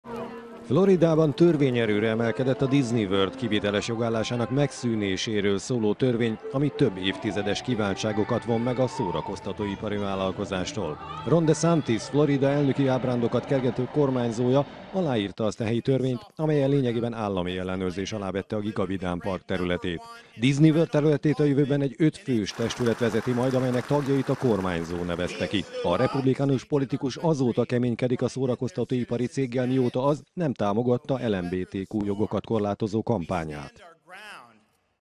Trump republikánus ellenfele harcba szállt a floridai Disney-park ellen
Elsősorban az eddig elengedett, óriási adóbevételek motiválták arra a politikust, hogy a szent tehénként kezelt park kedvezményeit megvonja.